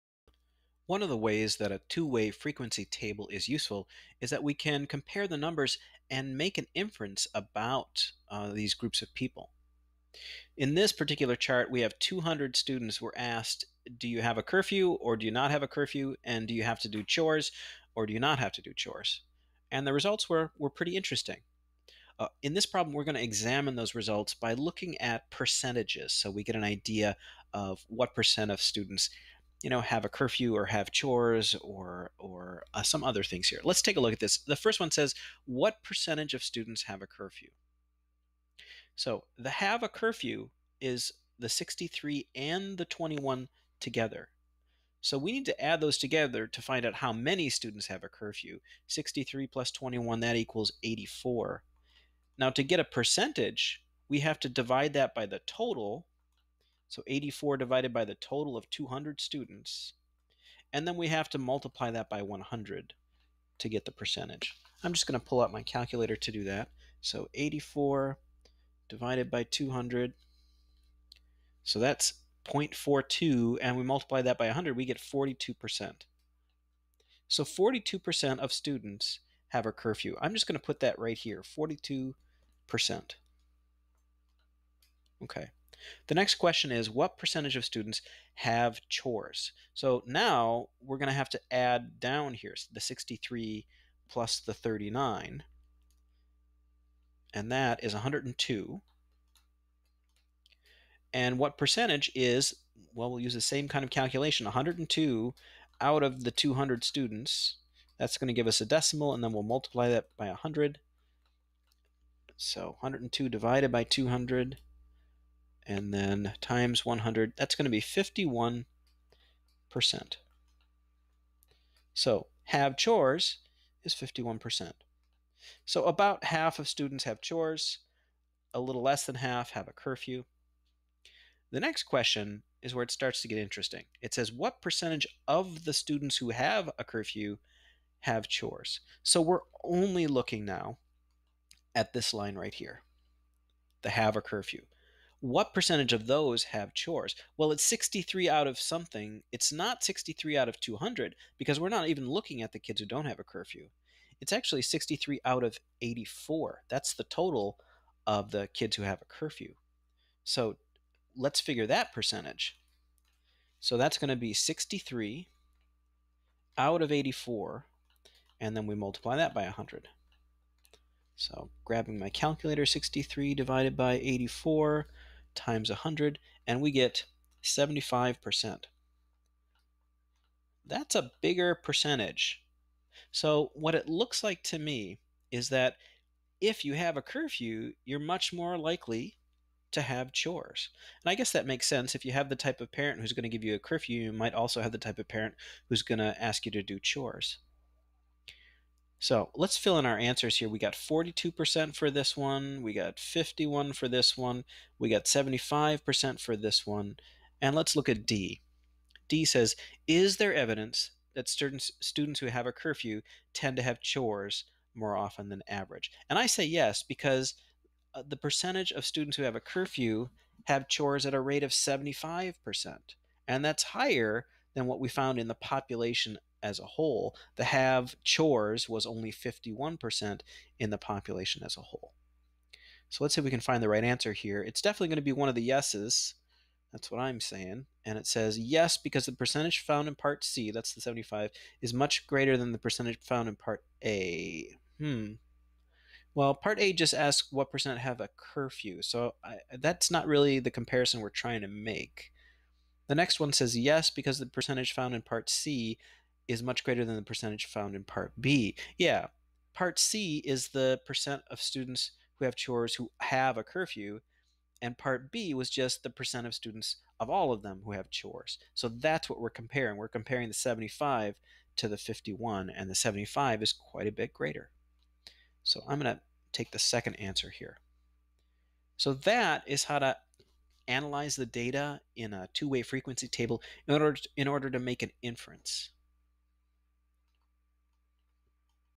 Making an inference using a two way frequency table
How to make an inference using a two way frequency table